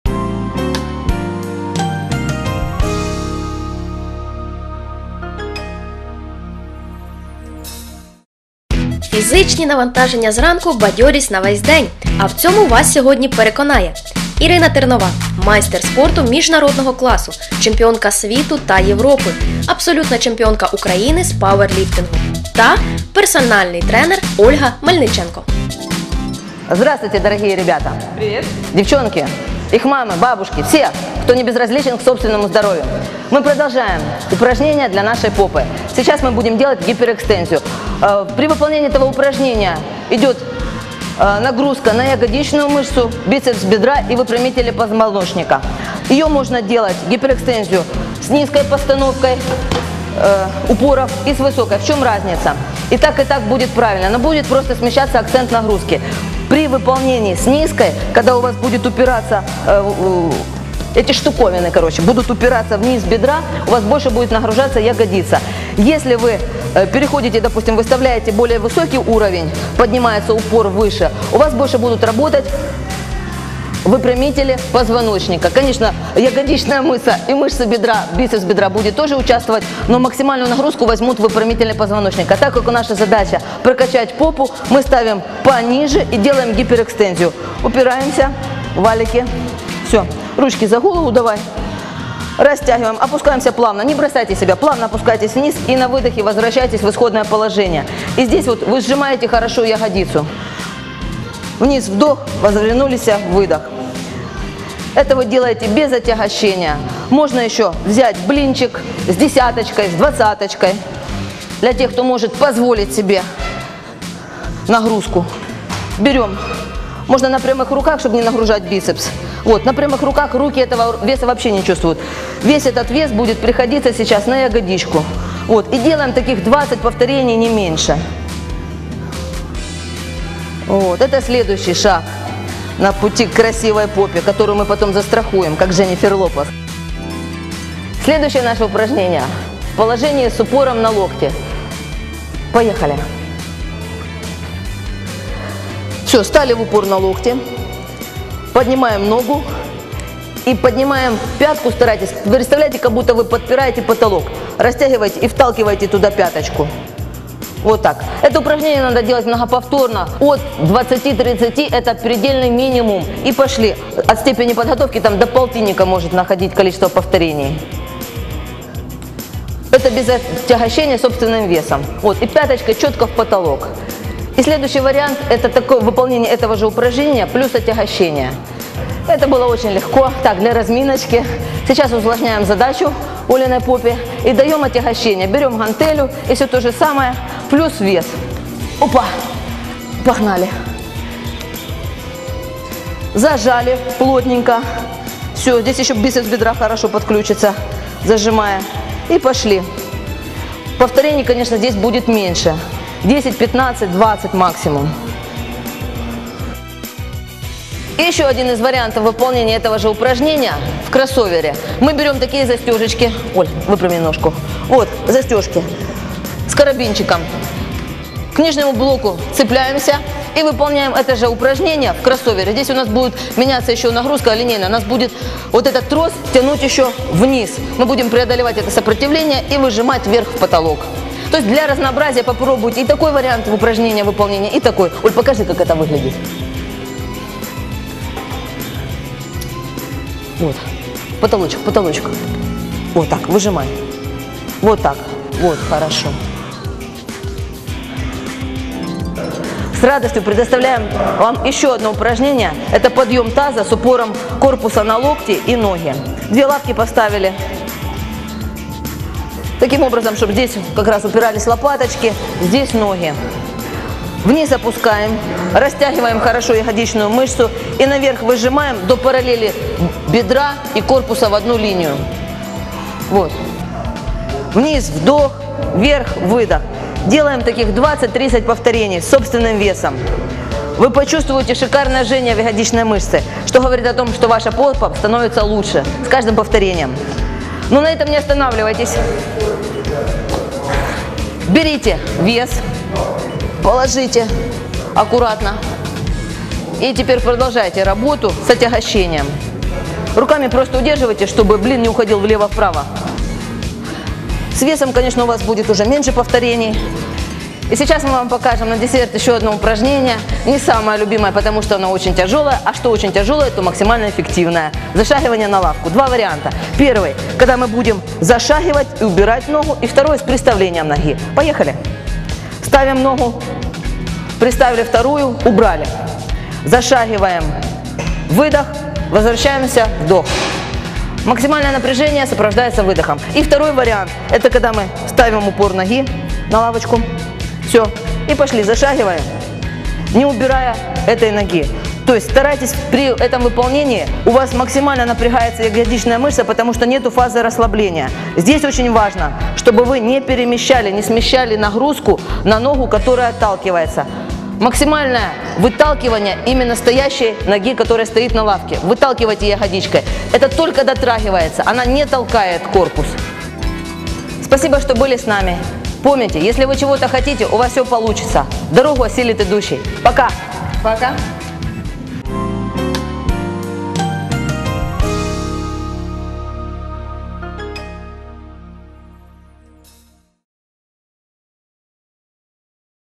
Новий день. Спорт у місті N. Вправи для м’язів сідниць
Комплекс вправ для тренування м’язів сідниць від сертифікованих персональних тренерів — Ірини Тернової та Ольги Мельниченко.